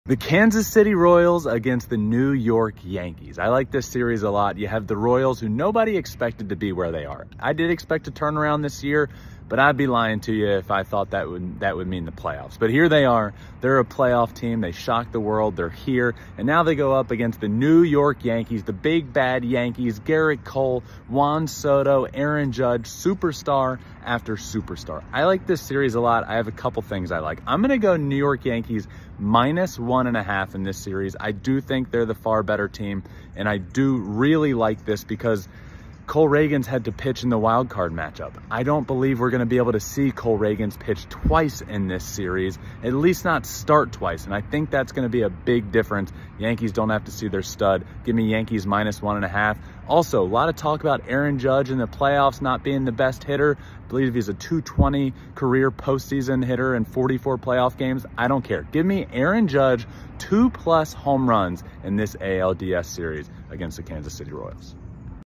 Ben Verlander PICKS the Yankees OVER Royals in the ALDS 👀⚾️
Ben Verlander Previews the ALDS matchup between the New York Yankees & Kansas City Royals.